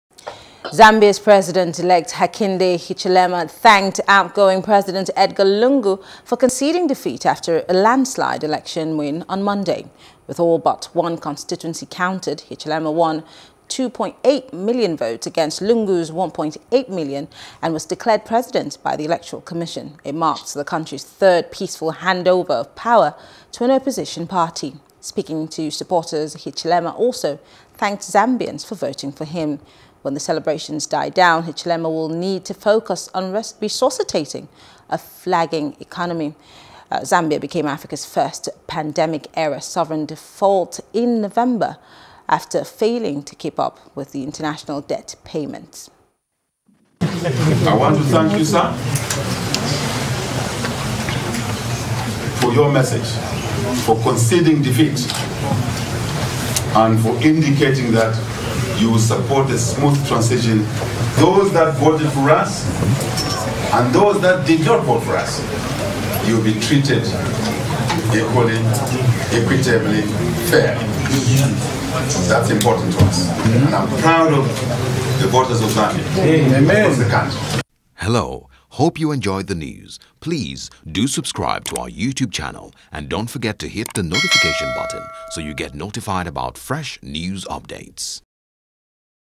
Zambia Election: President-Elect Hichilema Proud Of Voters | AFRICAN
Zambia's President-Elect Hakainde Hichilema thanked outgoing President Edgar Lungu for conceding defeat after a landslide election win on Monday.